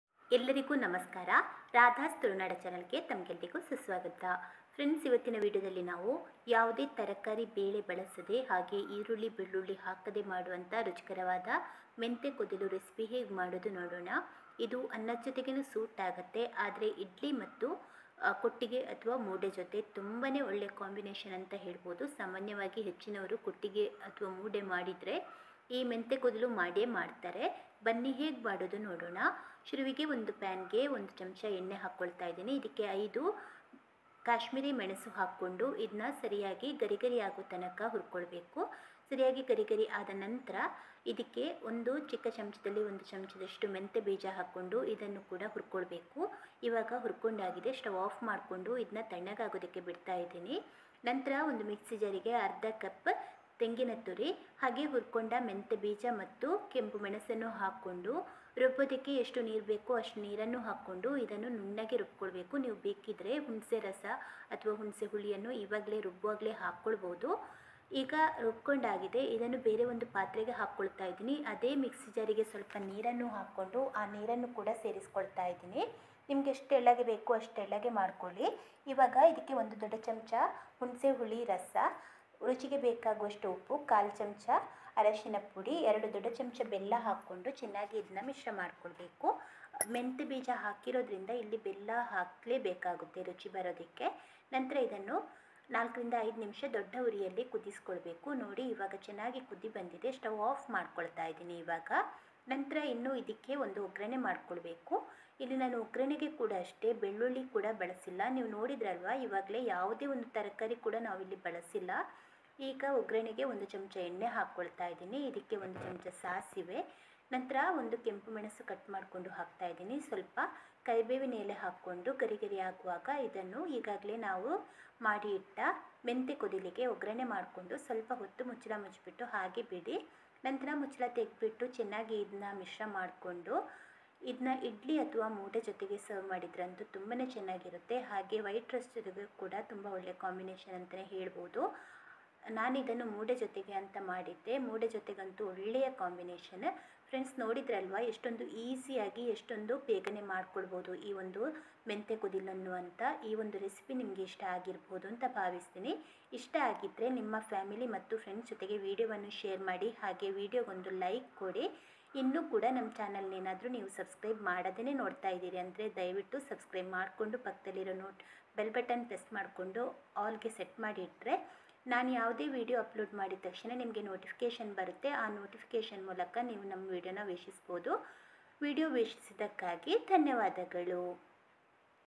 ಈರುಳ್ಳಿ ಬೆಳ್ಳುಳ್ಳಿ ಇಲ್ಲದೆ ಬೇಳೆ‌ ತರಕಾರಿ ಹಾಕದೆ‌ ಮಾಡುವ ರುಚಿಕರವಾದ ಮೆಂತ್ಯೆ ಕೊದಿಲು|Methi Rasam|ಮೆಂತೆ ‌ಸಾರು
ಈರುಳ್ಳಿ ಬೆಳ್ಳುಳ್ಳಿ ಹಾಕದೆ ಬೇಳೆ‌ ತರಕಾರಿ ಹಾಕದೆ‌ ಮಾಡುವ ರುಚಿಕರವಾದ ಮೆಂತ್ಯೆ ಕೊದಿಲು|Methi Rasam|ಮೆಂತೆ ‌ಸಾರು